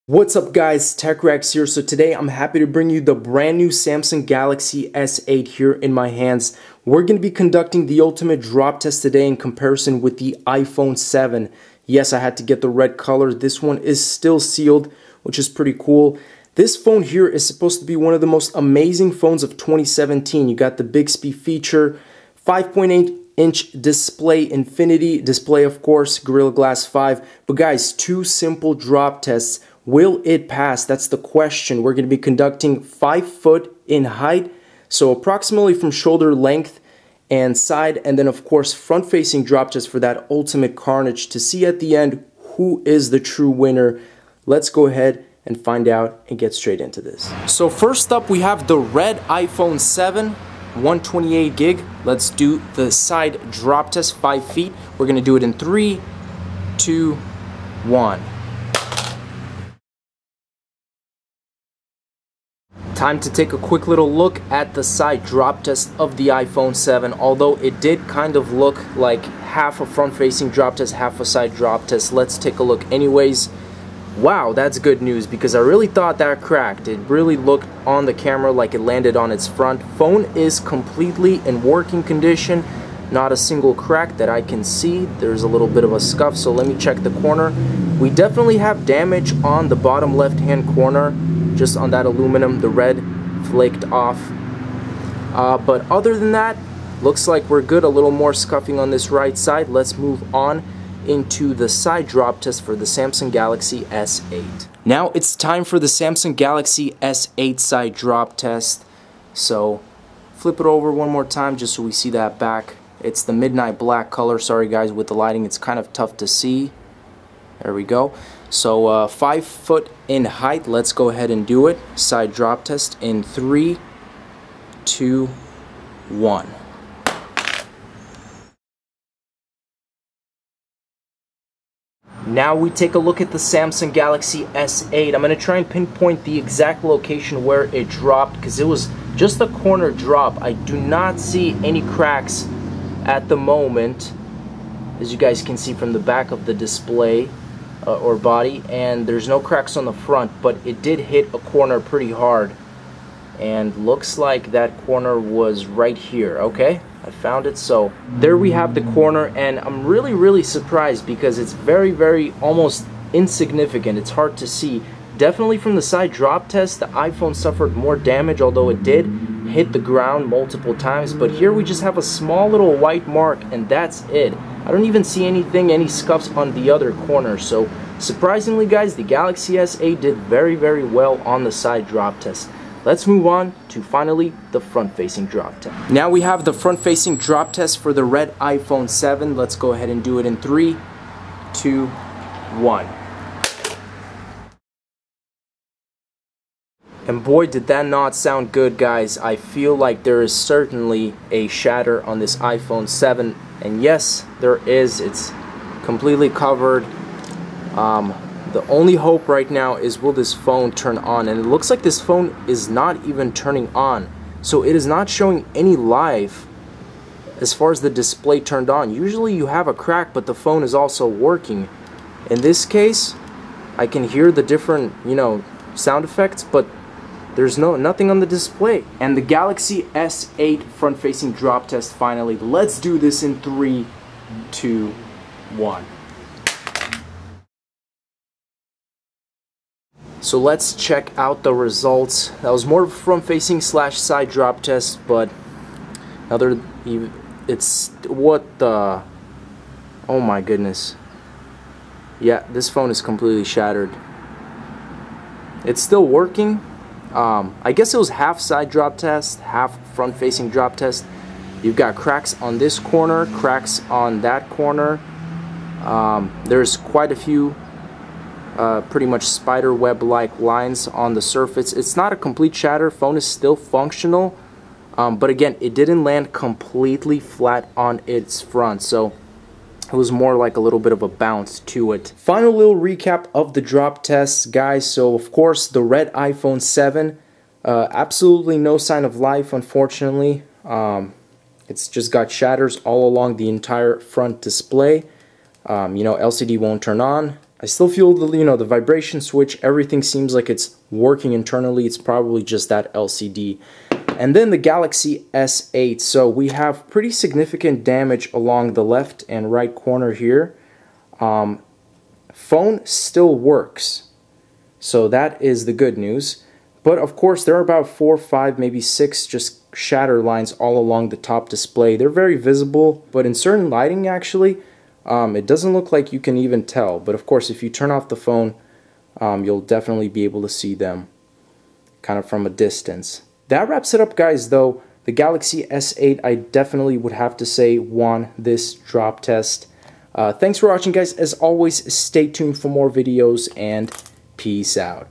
Samsung Galaxy S8 vs iPhone 7 Drop Test!
Samsung Galaxy S8 or the iPhone 7? Which flagship device is the winner in this drop test?